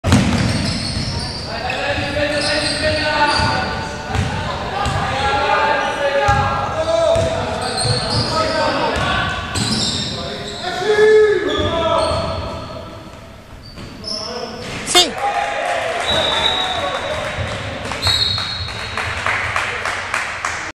2012/13_SanMarco vs Albignasego_Lombardo con la bomba
Ultimo canestro della gara grazie alla tripla dall'angolo del cecchino Lombardo.